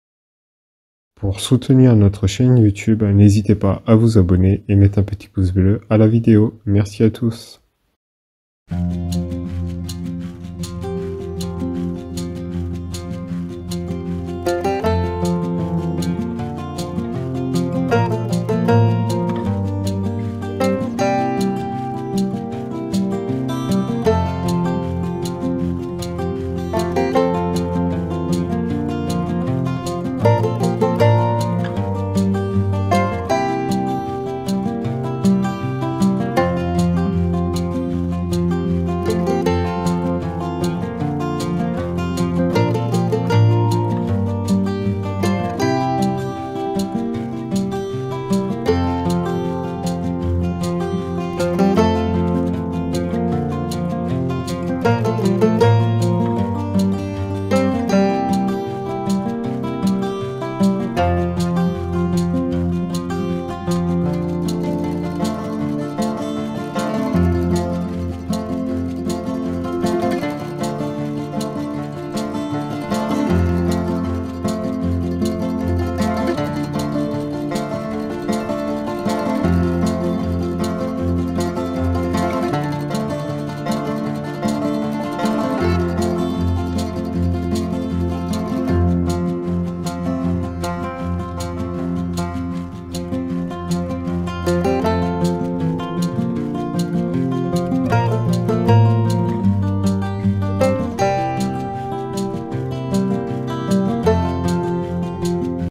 🥗 Salade de Riz, Jambon-Petits Pois – Un Classique Familial Rapide et Gourmand
La salade de riz au jambon et petits pois est une recette simple et efficace, parfaite pour un repas rapide et équilibré. 🍚🥓 Avec sa touche de fraîcheur et sa gourmandise, elle ravira petits et grands. Découvrez comment préparer cette salade incontournable en toute simplicité !